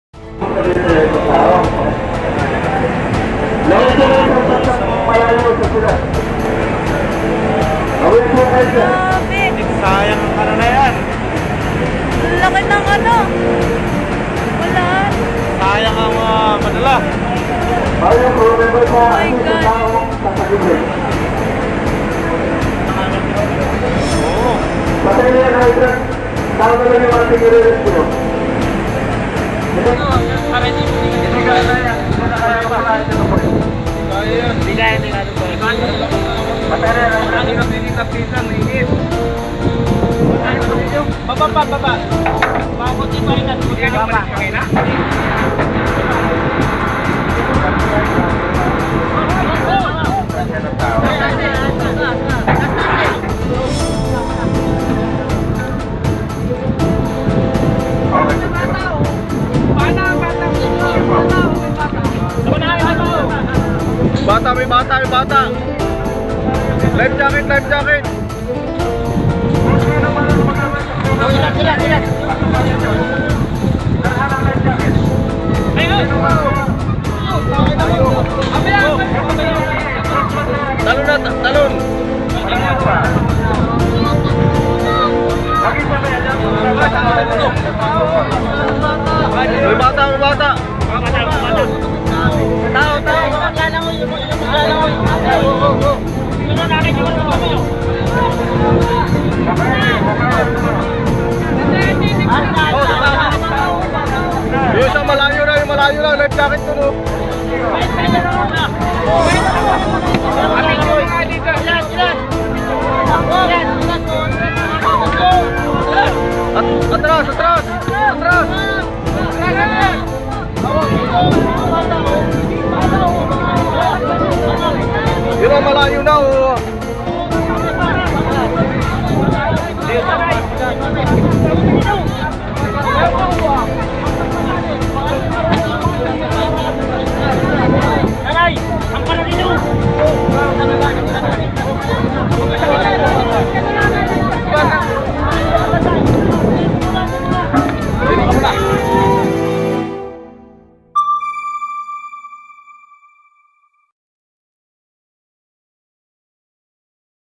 Kapal Feri dengan 134 Penumpang Terbakar di Perairan Filipina, 7 Tewas
Kapal feri berkecepatan tinggi angkut 134 penumpang terbakar, Senin (23/5/2022).

Android: inews.id/android
iPhone: inews.id/ios